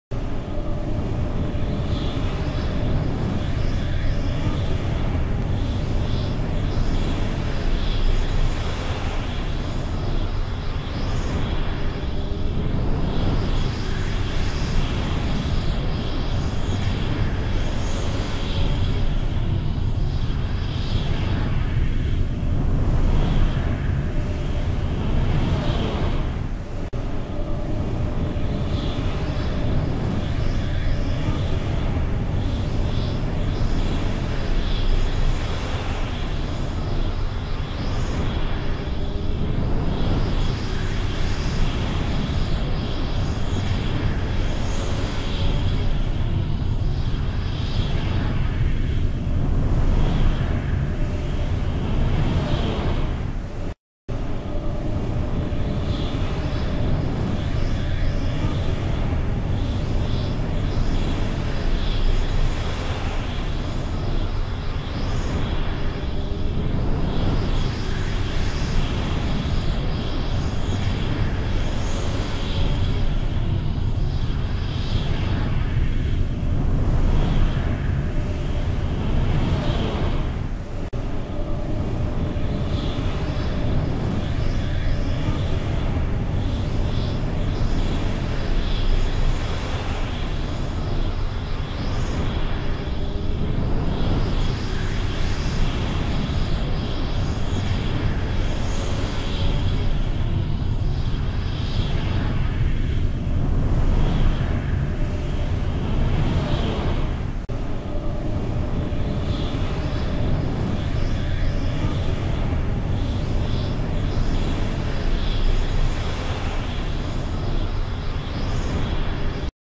Galaxy view and Astrology Signs ans Stars
See the Galaxy views and Astrology Sings ans Stars. Sun family